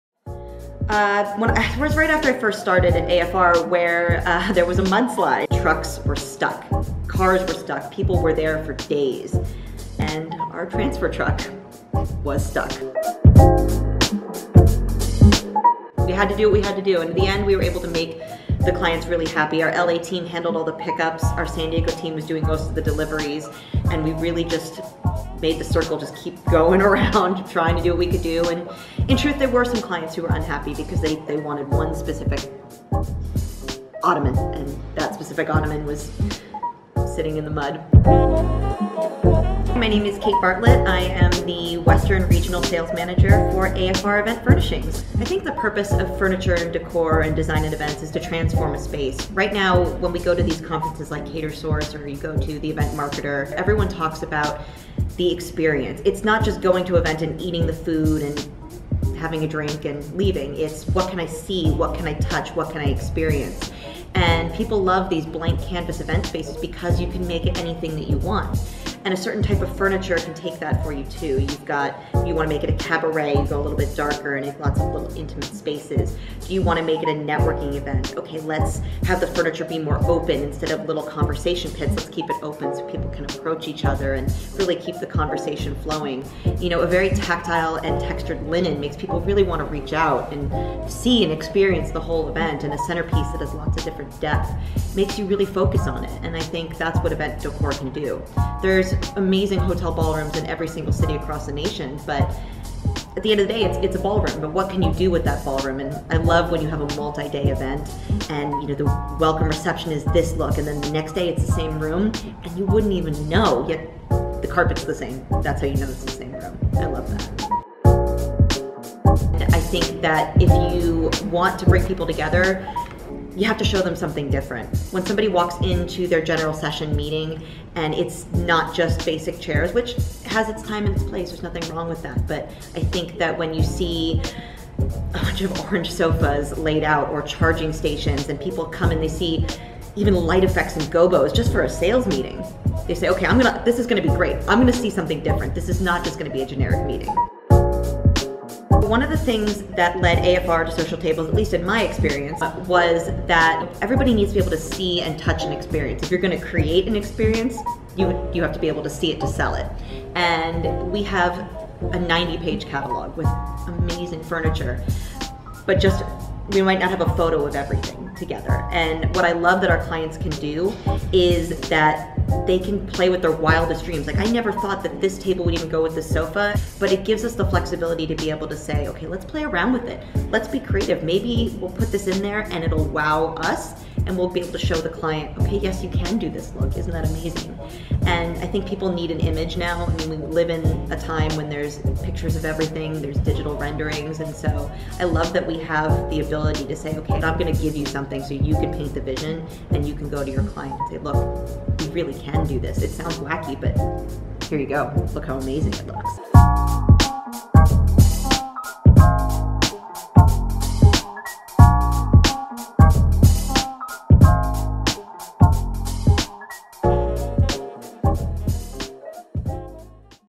How to Transform Event Venues with Furniture - Kate Bartlett, AFR Event Furnishings
What's the purpose of event decor? Kate Bartlett of AFR Event Furnishings shares how good design can transform any event venue.

If you're like us, then you love nerding out over events and hospitality.